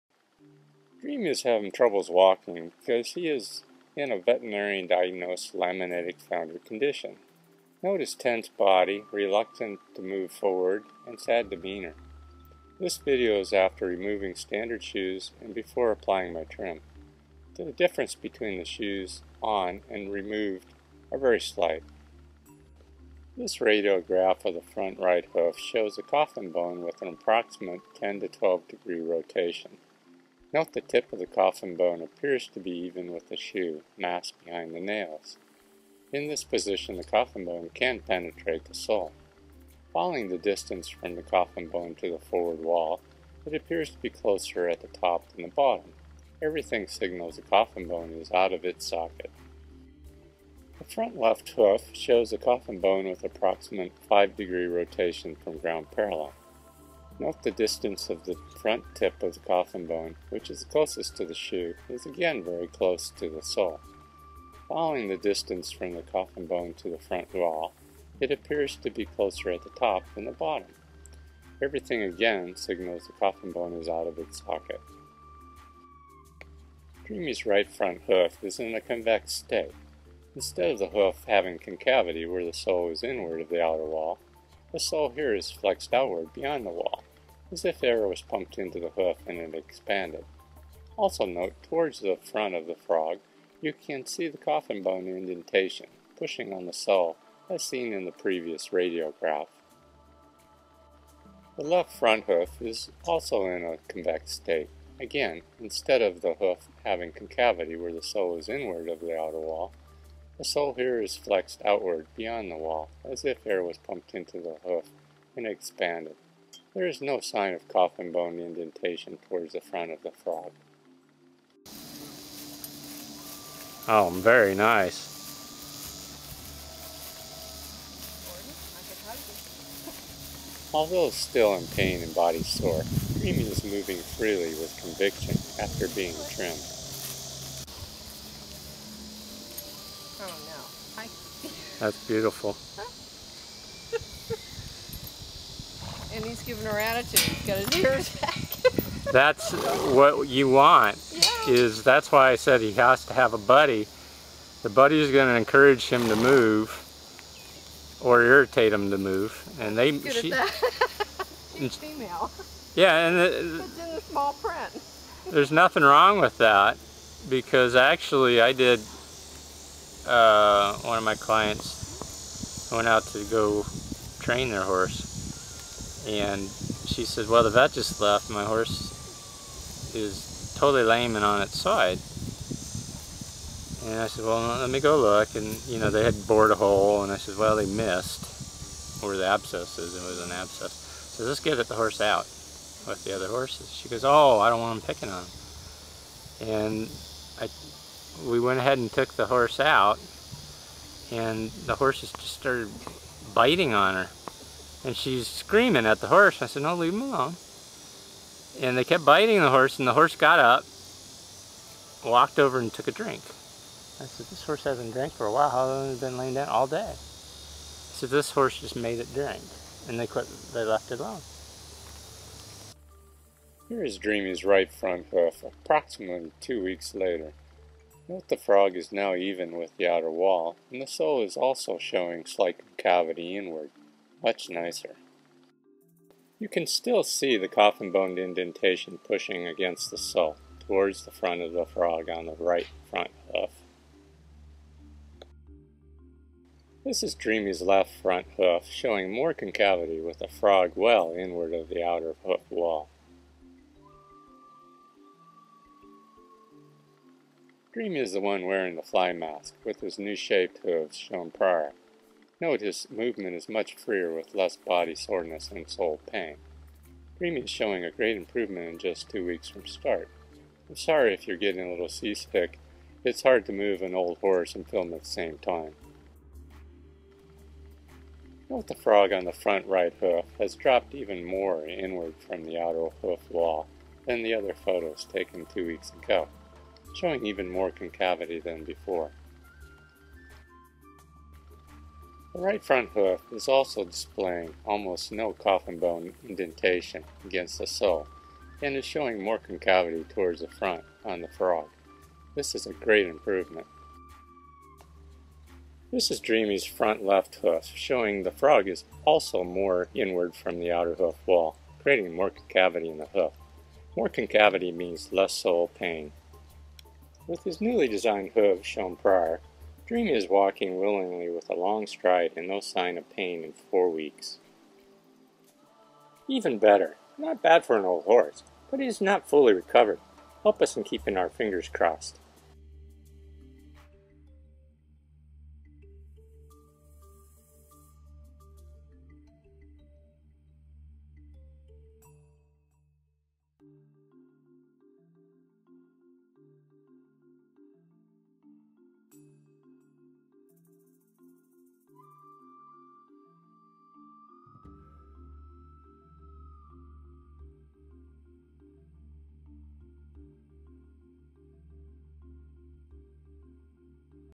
Swollen sole and laminitis / founder - Dreamy
Swollen hooves to increased concavity, coffin bone sole pressure, and video movements, three stages in four weeks. Dreamy's recovery from a laminitis / founder condition

Gordon Adair Natural Barefoot Horse Trimming
Citra / Ocala, Florida